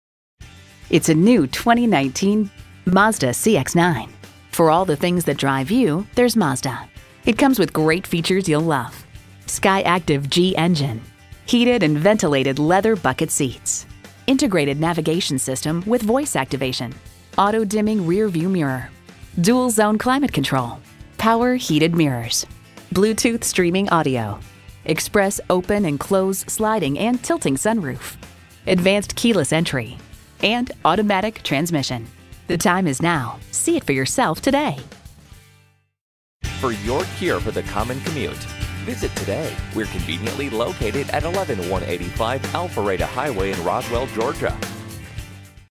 New 2019 Mazda CX-9 Roswell GA Atlanta, GA #313756
Year: 2019
Make: Mazda
Model: CX-9
Trim: Grand Touring
Engine: 2.5L 4-Cylinder Turbocharged
Transmission: 6-Speed Automatic
Color: Gray
Interior: Black
Stock #: 313756
VIN: JM3TCADY1K0313756
Price includes: $1,000 - Customer Cash. Exp. 01/31/2019 Recent Arrival! $2,416 off MSRP! Machine Gray Metallic 2019 Mazda CX-9 Grand Touring FWD 6-Speed Automatic 2.5L 4-Cylinder Turbochargedbrbr22/28 City/Highway MPG

Address:
11185 Alpharetta Hwy